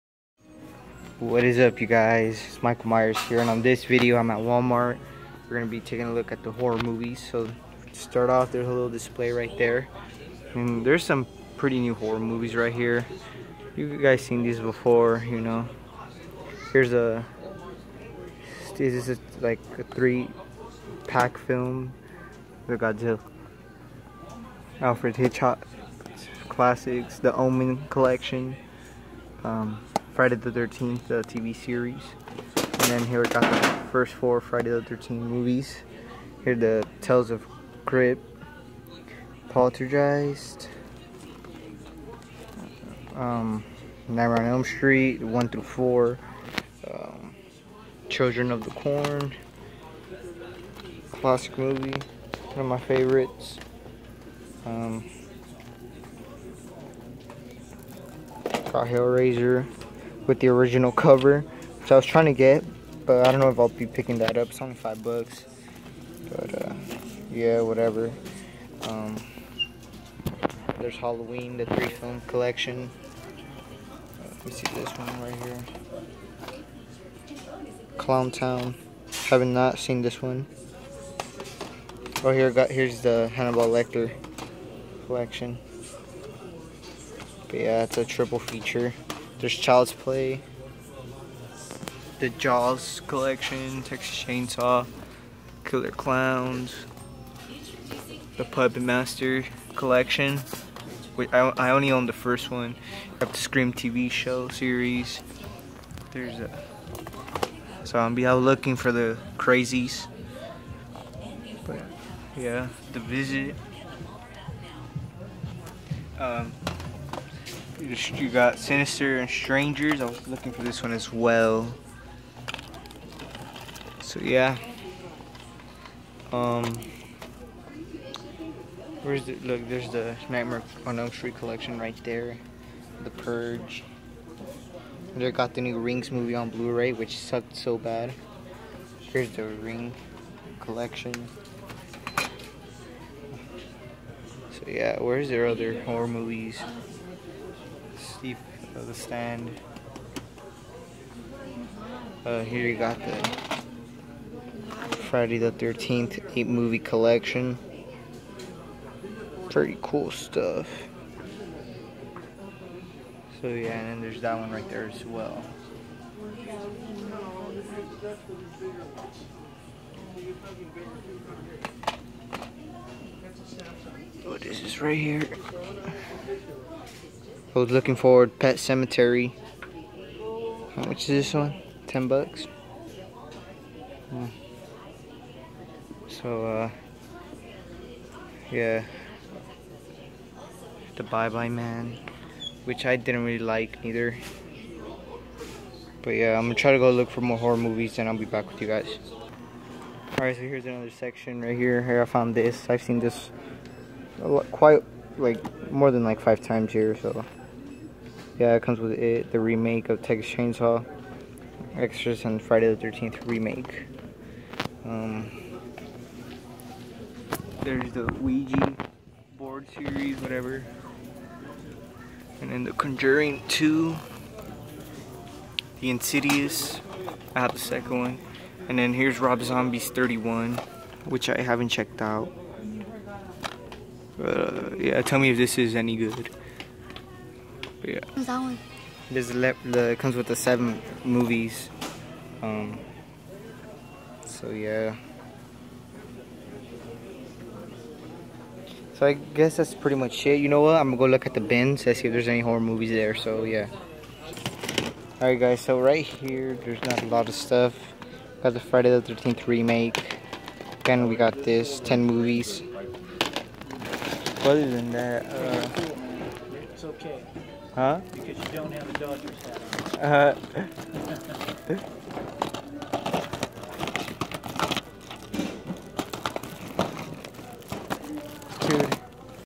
Horror DVDS At Walmart
The video got cut short but these were all horror films at Wal-Mart